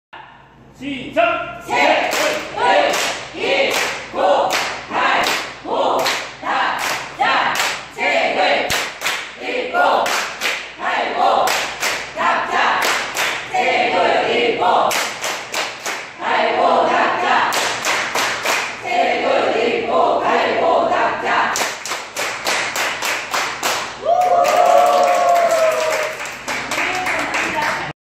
대구독서모임 달서독수리 책박수, 김정원강사
대구 달서구에서 진행하는 최강독서모임 달서독수리 클로징, 독수리박수